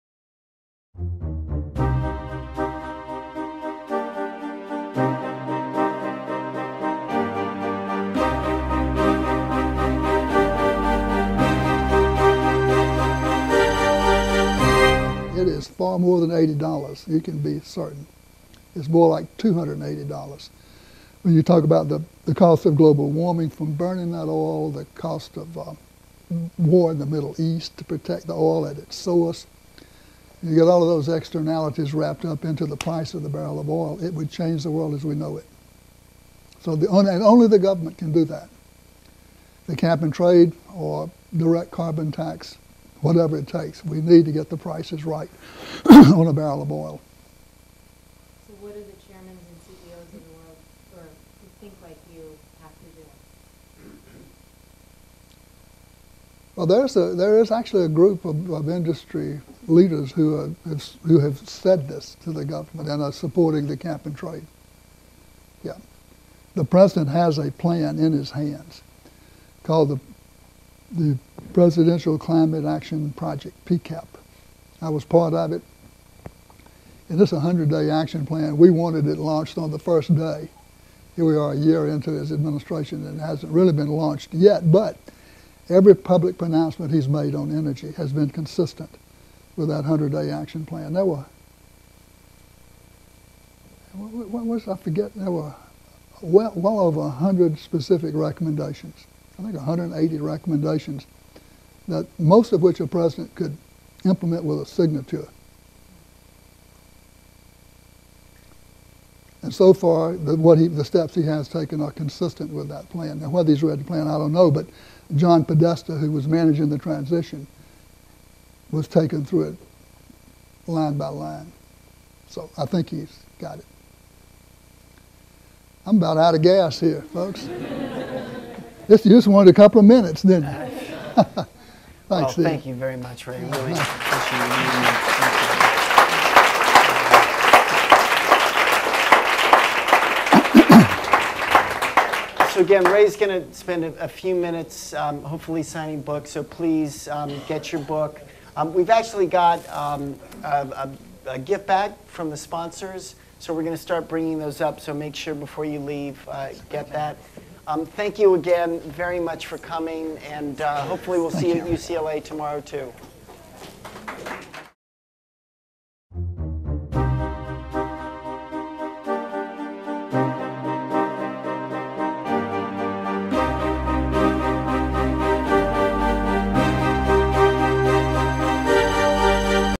Green Rush MicroDoc 6
Ray C. Anderson, Founder & Chairman, Interface, Inc. concludes his talk with discussing the "100 Day Action Plan," and Steve Glenn, CEO, LivingHomes, & Chair, Board of Directors, Sustainable Business Council thanks everyone!